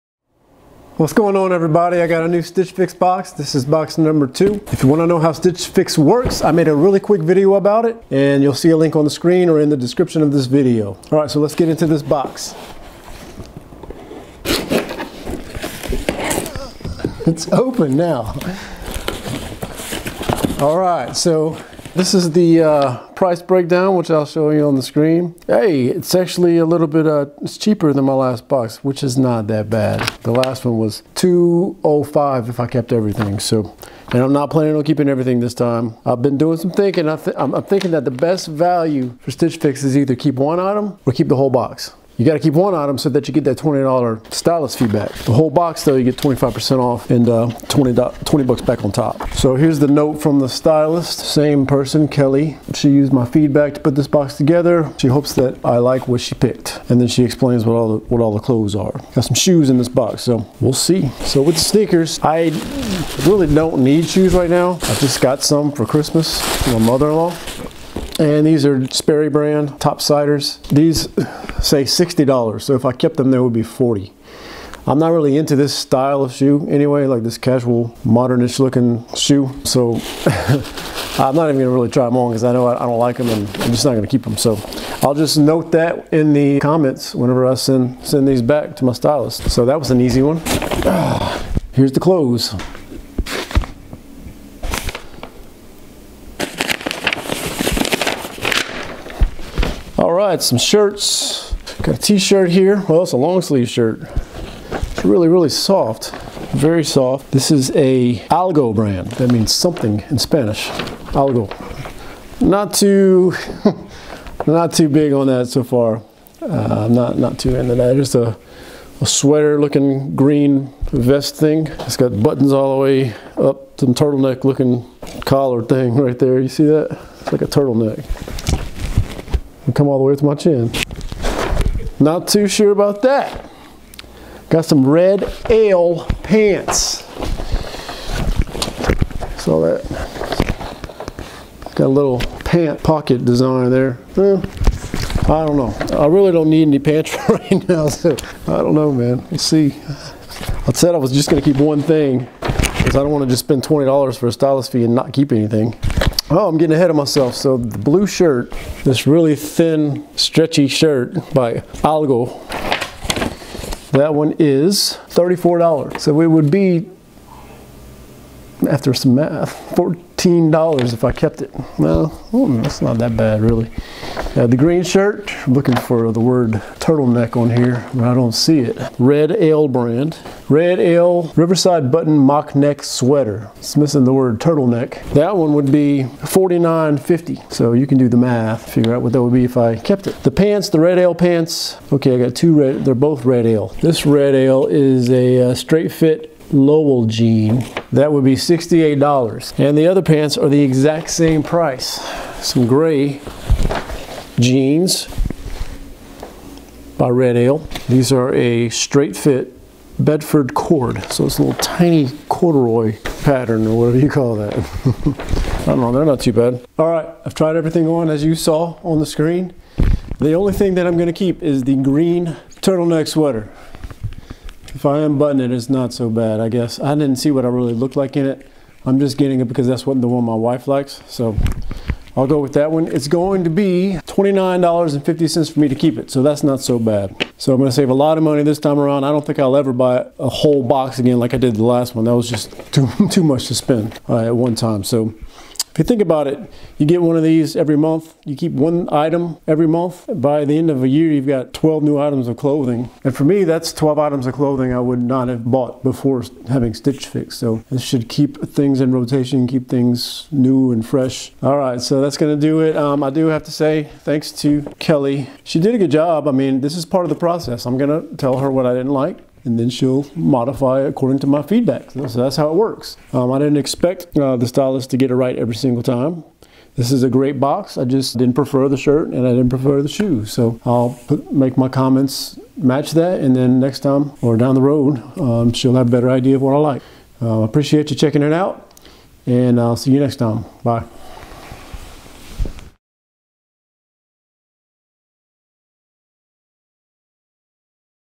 StitchFix Men // My Second Box
This is the second box I've received from Stitch Fix.